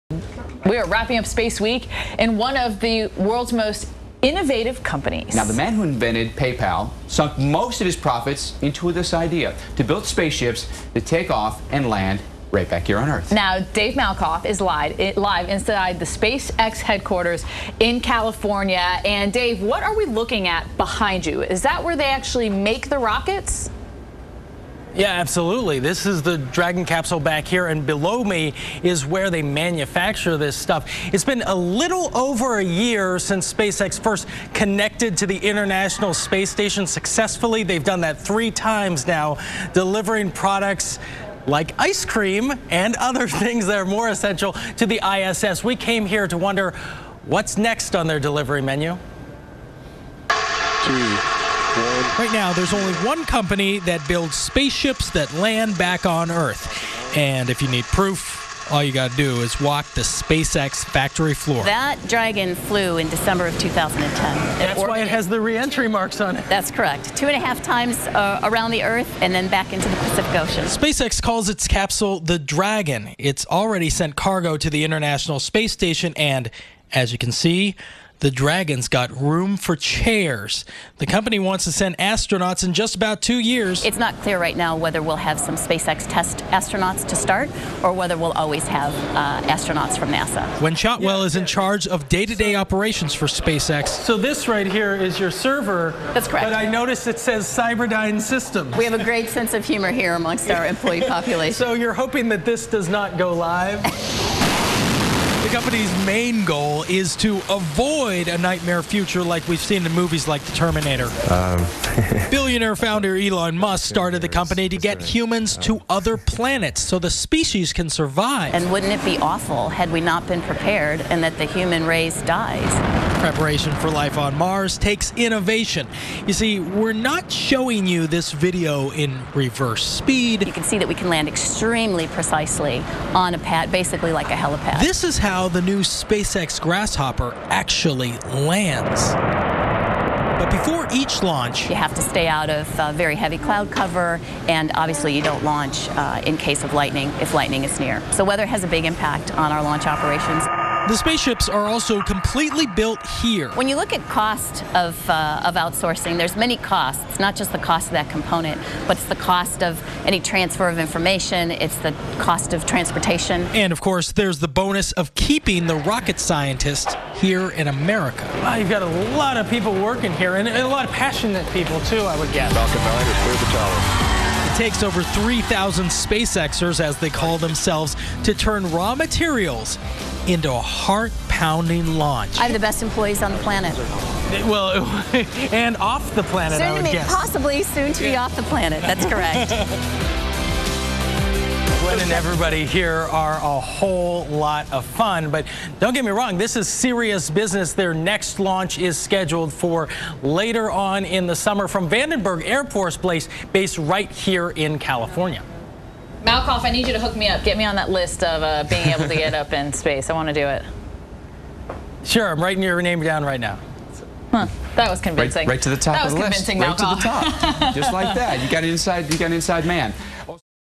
Inside SpaceX Headquarters
A rare look at where the Falcon 9 Rocket & Dragon Capsule are constructed in Southern California. SpaceX COO Gwynne Shotwell shows reporter Dave Malkoff ] around.